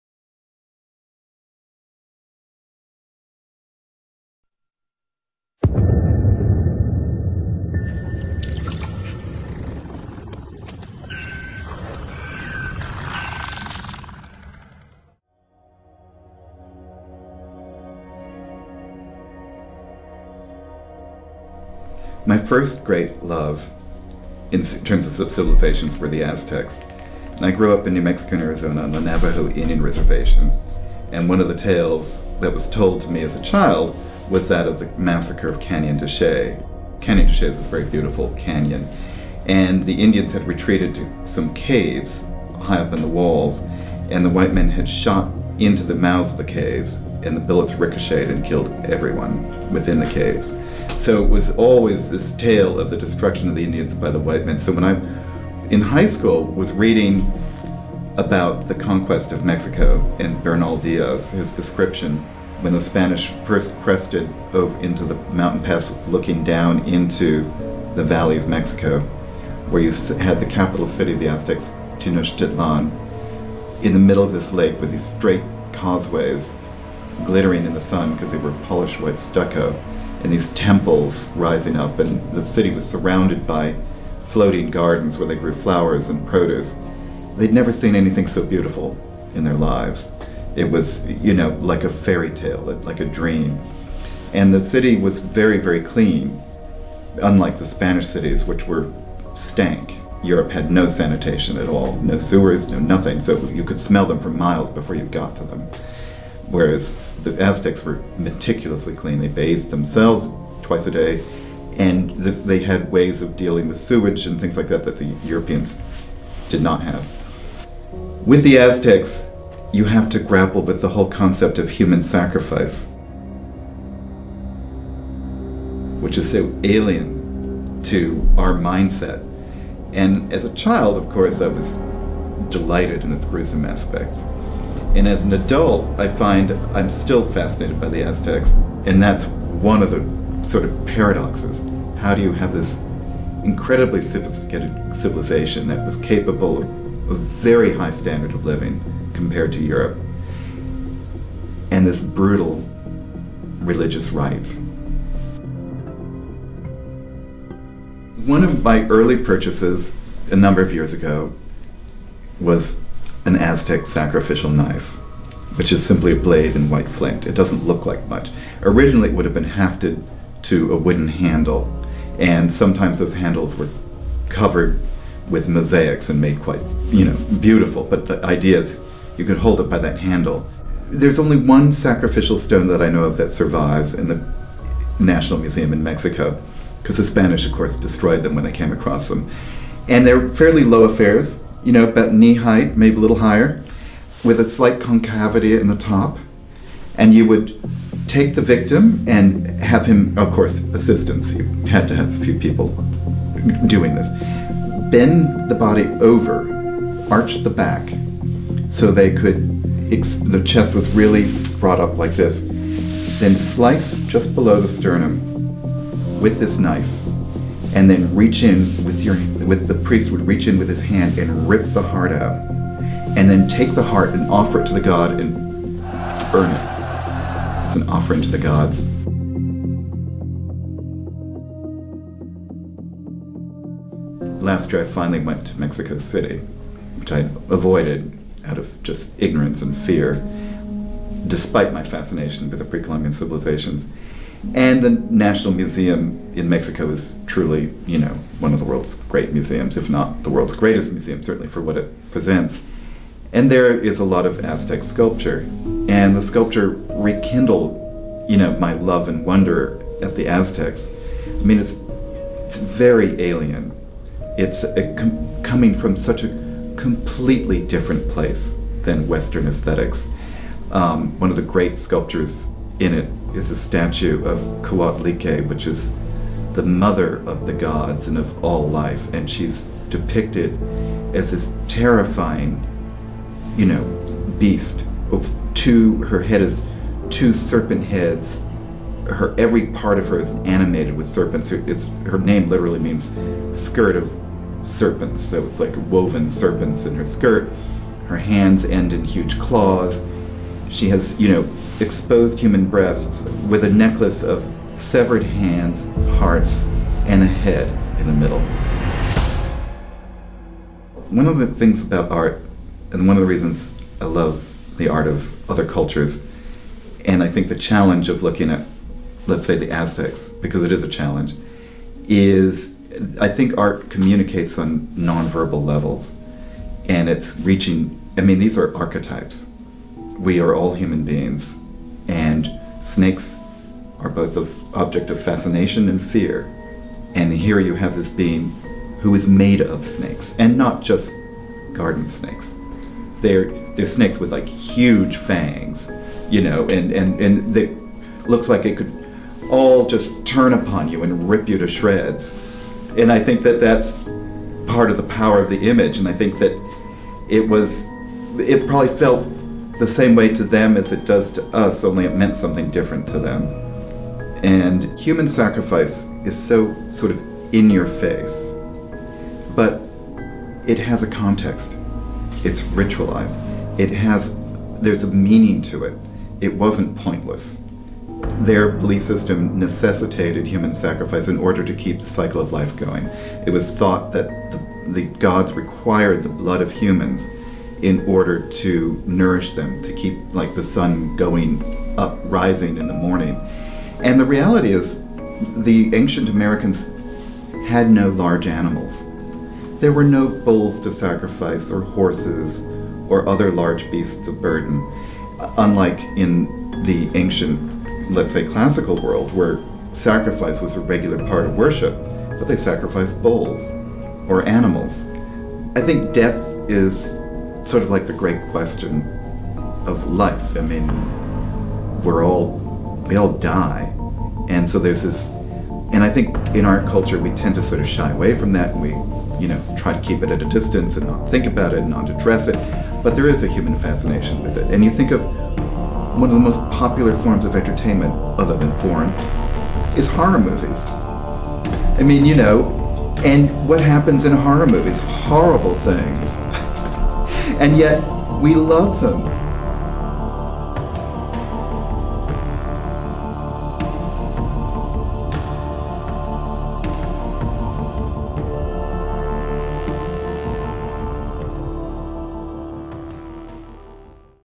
Paradox of the Aztecs
Thinking about the Aztecs, paradoxically civilized and brutal.